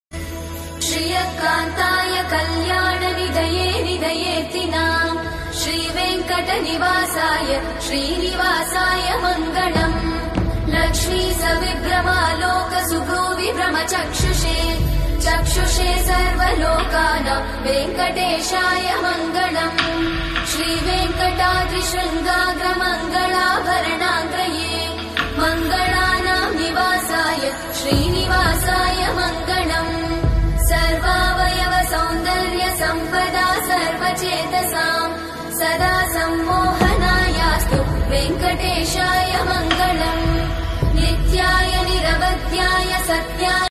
⚡⚡Super New Design Casting Sri Venkateshwar Swamy Ring || Trending Gold Ring || October 2022 Design
Size 👉 24 & 26 Numbers
Purity 👉 916/22 Karat Purity
Price 👉 1lac

Made On Customer Order,DM Me To Order!!

Ambica Jewellers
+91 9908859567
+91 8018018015